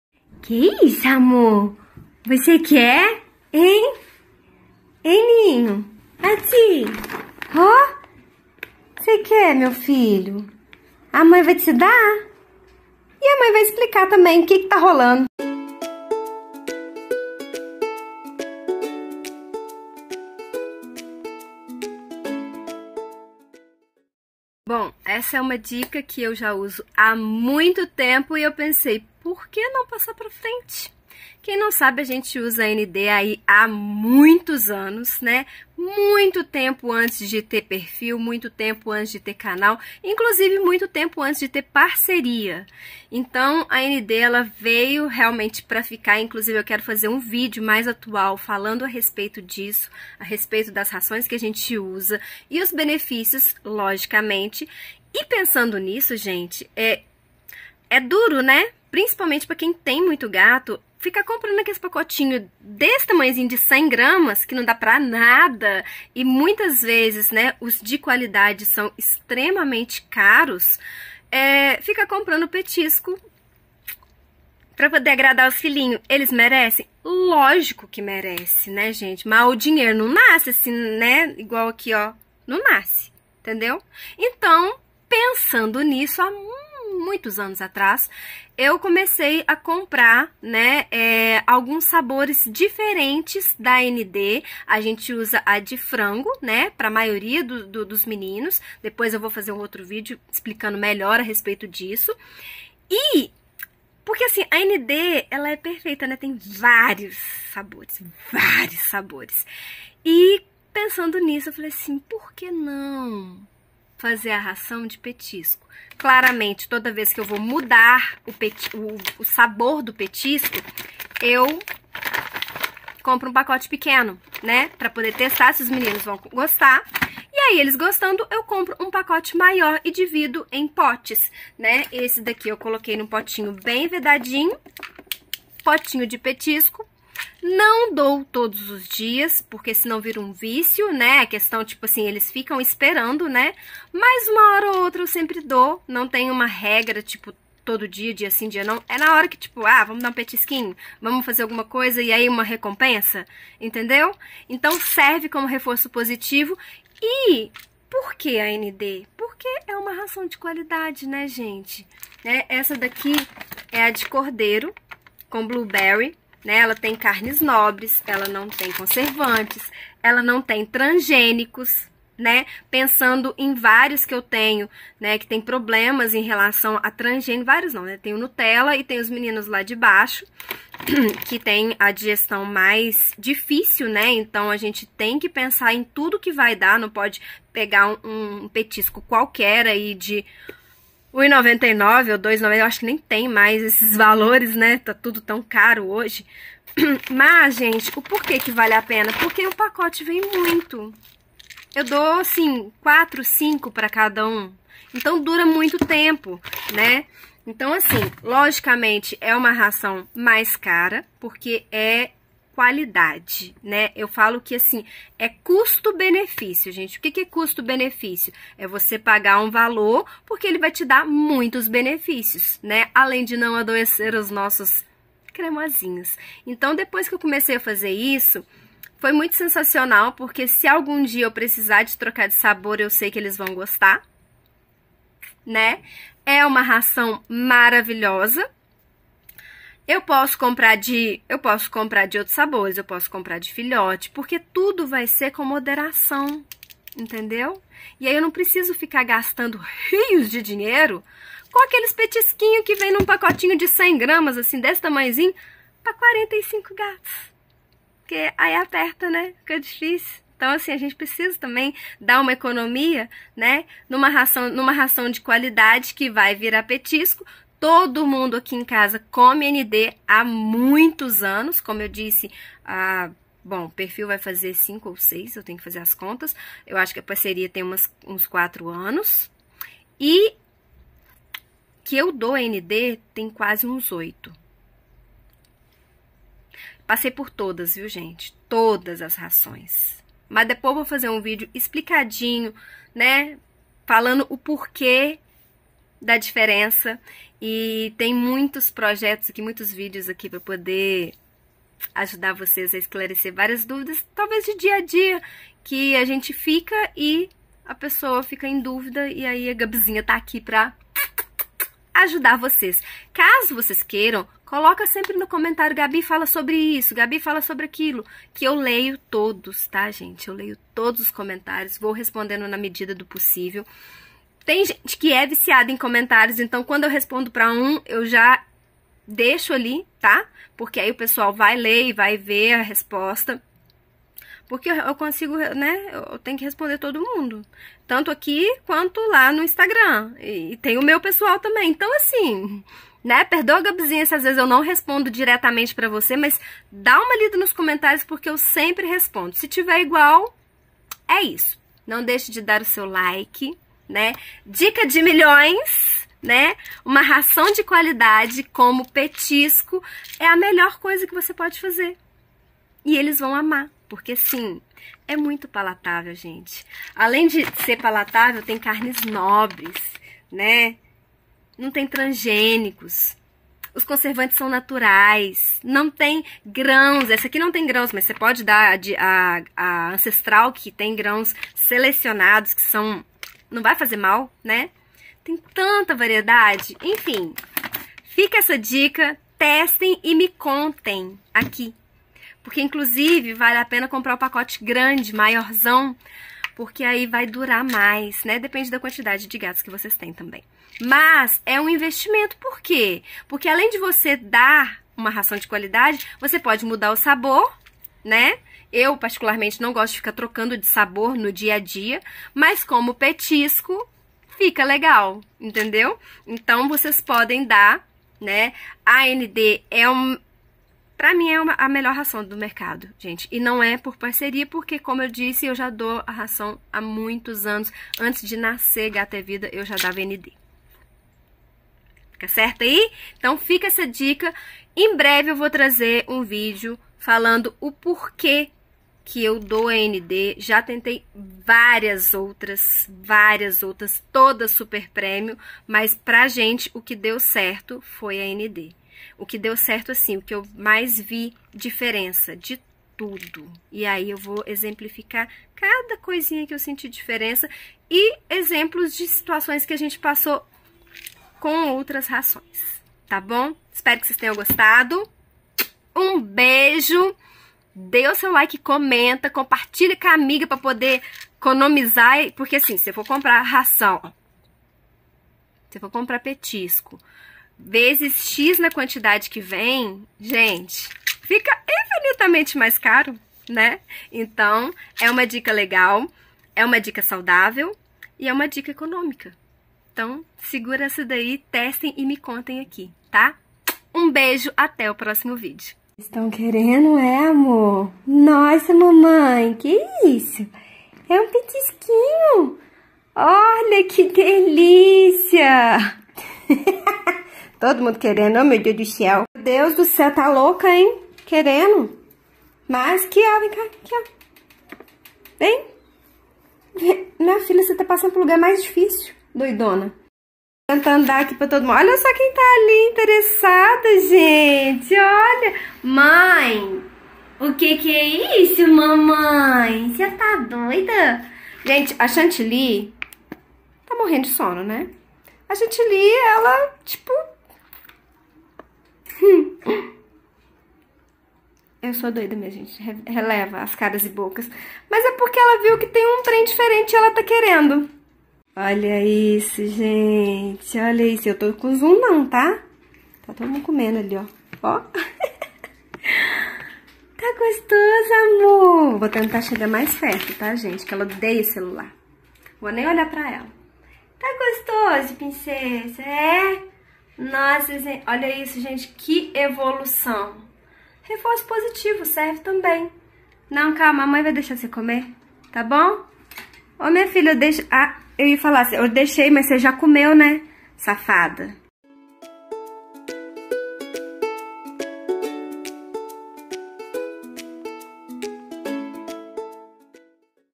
Melhor petisco para gatos | Gato é Vida
Que nossos cremosos amam petisco é indiscutível, mas que trem caro que acaba rápido!

E foi assim que tive essa excelente idéia e passei a conseguir agradar muito mais meus filhos, de uma forma mais econômica e saudável!
Então! 🤪🙈🥰

Corre pro canal para ver tudinho!!!🥰

Muitas dicas, informações e histórias dos nossos cremosinhos!

Acesse ou clique no link da Bio.

Lambidinhas cremosas!